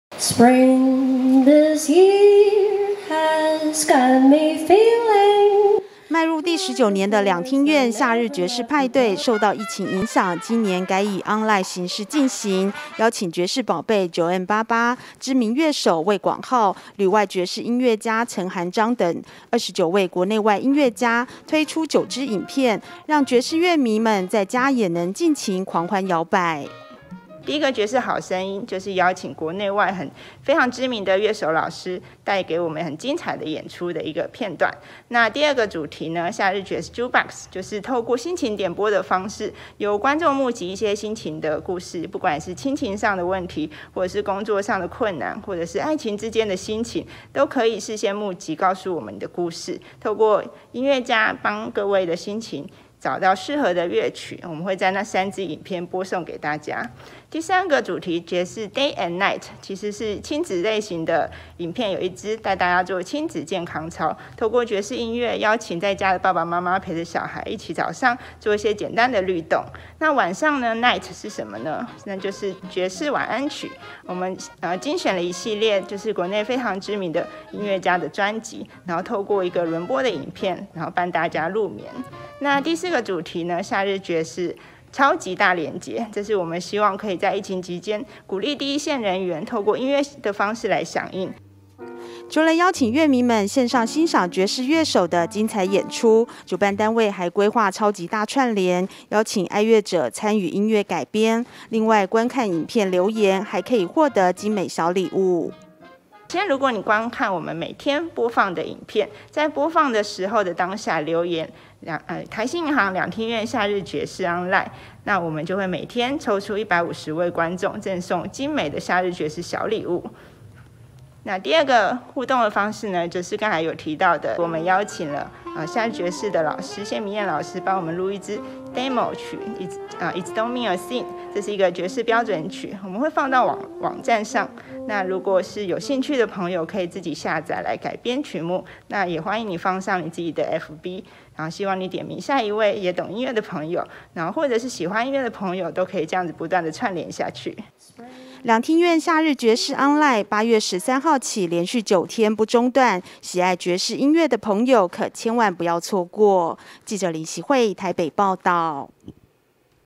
爵士音樂|兩廳院夏日爵士ONLINE 在家狂歡搖擺
深受爵士樂迷喜愛的「兩廳院夏日爵士派對」，今年改以ONLINE形式進行，邀請國內外29位爵士音樂家，推出9支網路影片，還有心情點歌機與愛樂者超級大串聯，邀請爵士樂迷一起在家狂歡搖擺。